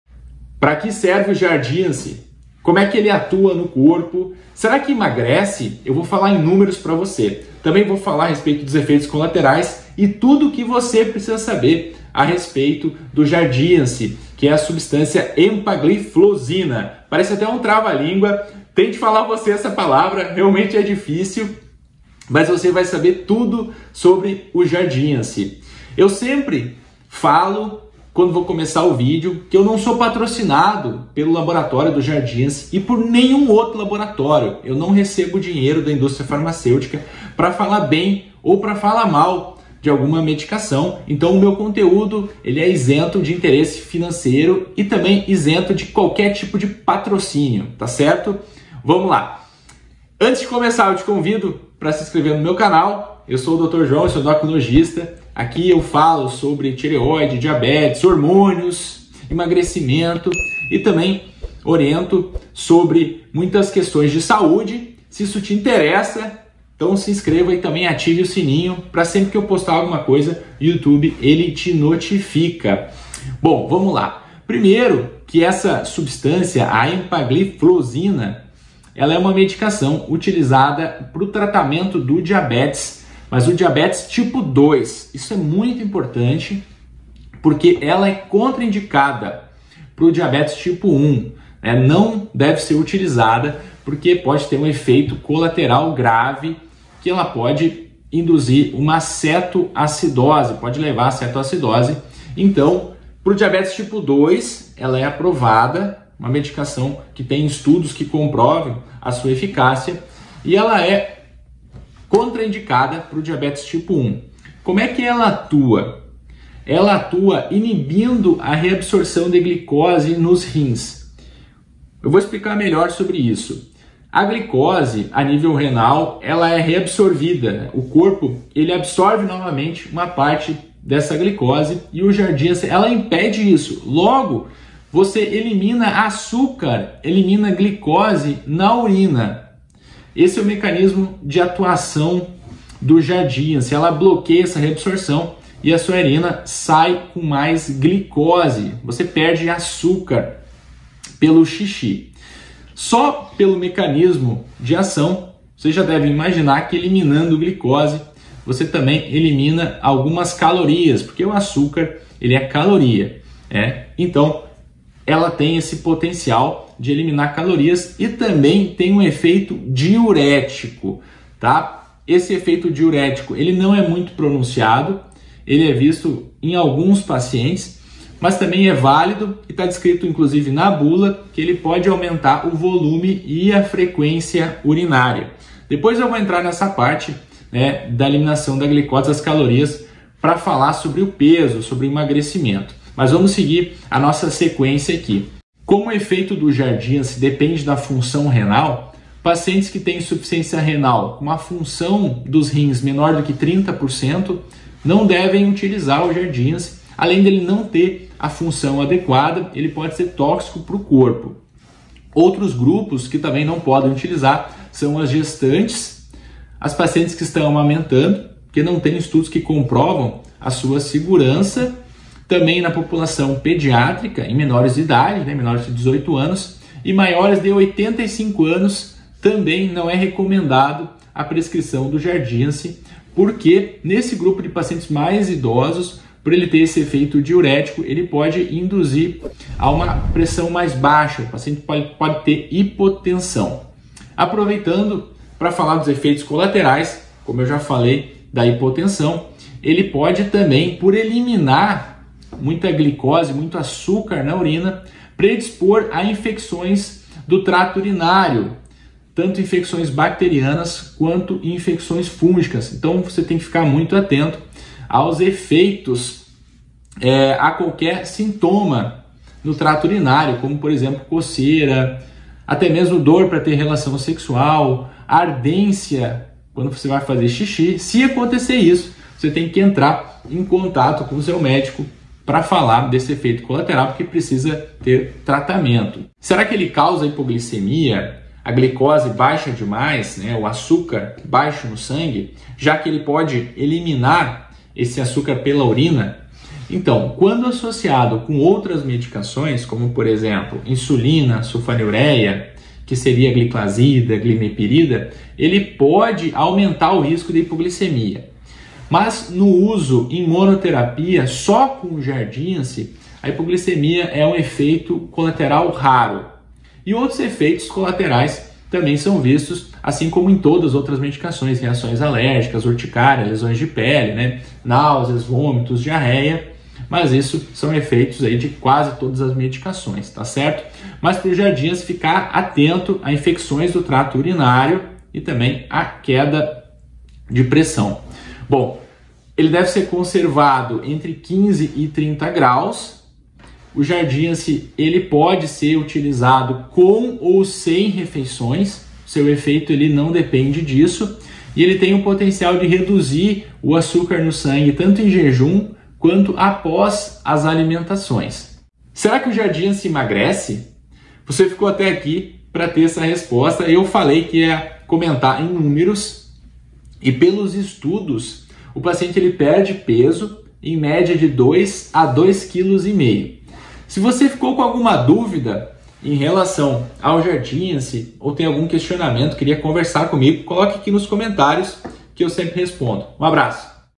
Jardiance emagrece? Para que serve? Bula explicada
Jardiance emagrece? Para que serve? Tem efeito colateral?
Jardiance bula explicada pelo endocrinologista Dr. João
Como funciona o jardiance 25mg e 10mg?
Será que causa hipoglicemia e quantos quilos o jardiance emagrece?
O nome genérico (da substância) é a empagliflozina.
Hipoglicemia seria um dos efeitos colaterais?

Índice do vídeo:
0:00 Inscreva-se
0:15 Introdução
0:31 Para que serve o Jardiance? Emagrece? O que diz na bula?